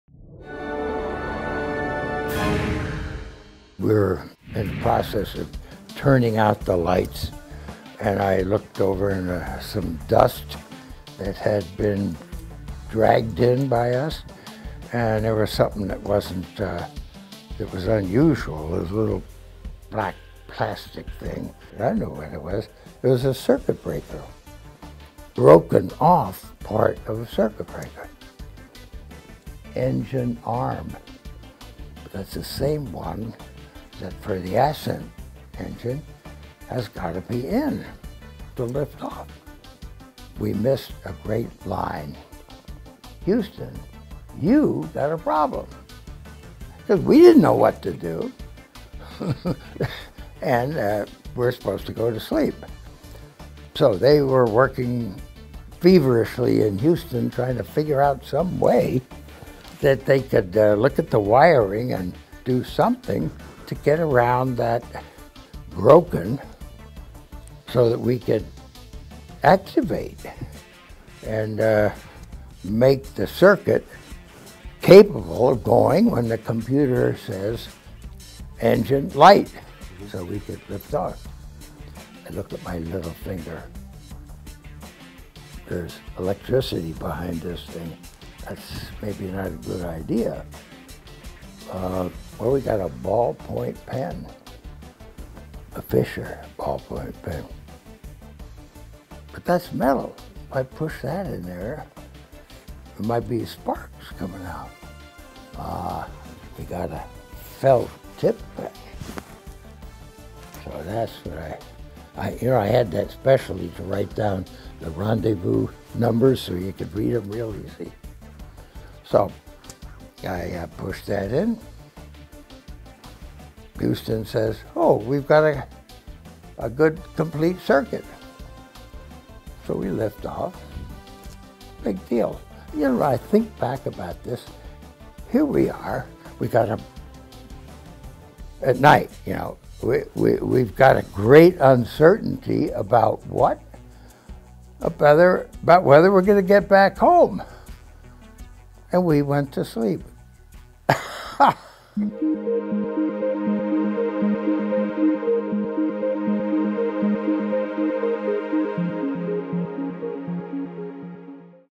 Tell Me A Story: Buzz Aldrin's Felt Tip Pen Saves Day
Apollo 11 astronaut Buzz Aldrin tells the story of when he used quick thinking and a felt tip pen to repair a broken circuit breaker inside the lunar module... while on the surface of the Moon.